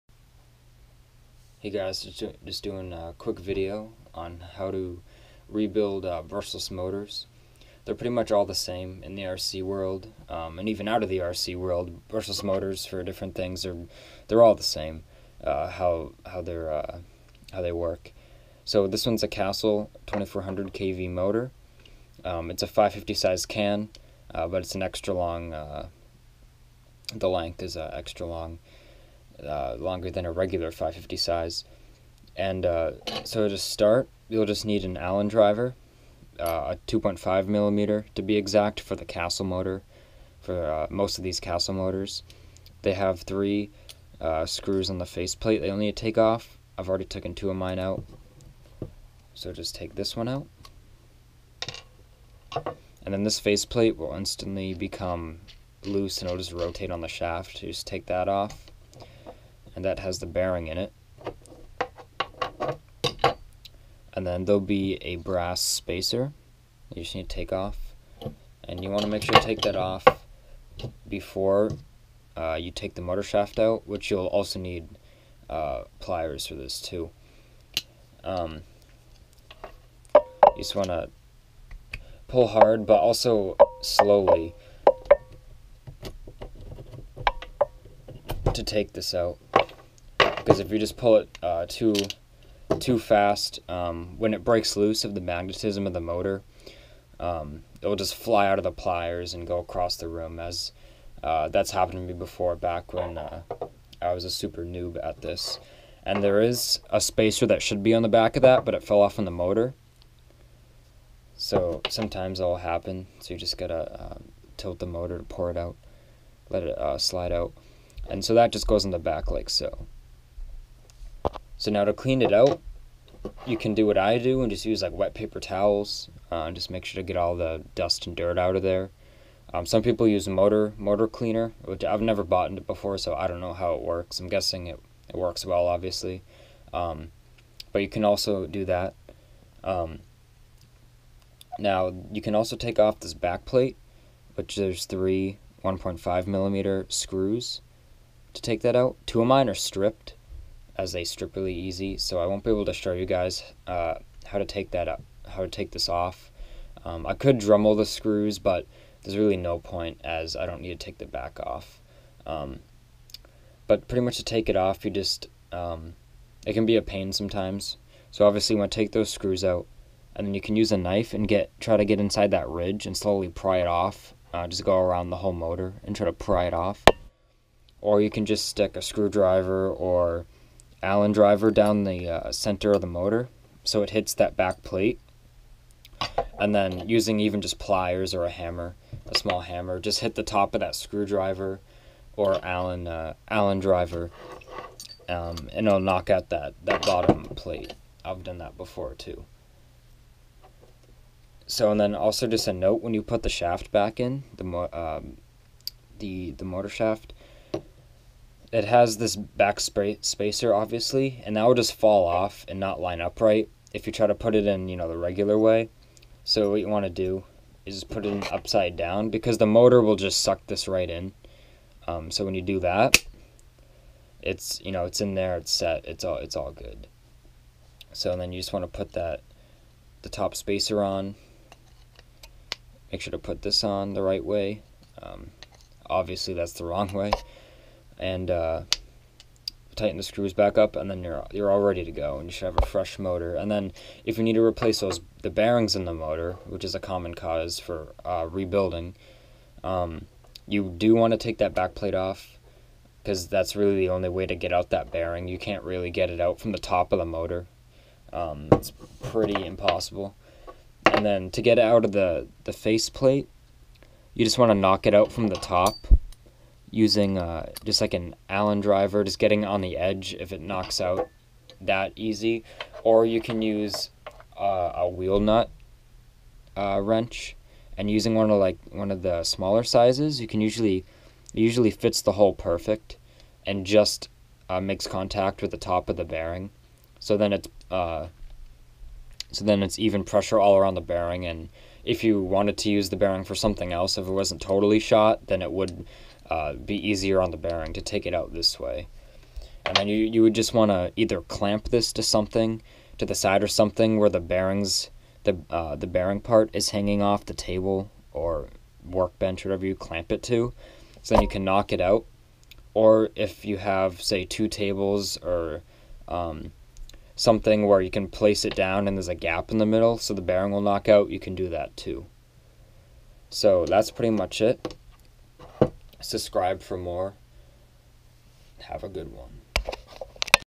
How To Rebuild A Brushless Motor
Just a quick video on how to rebuild a brushless motor.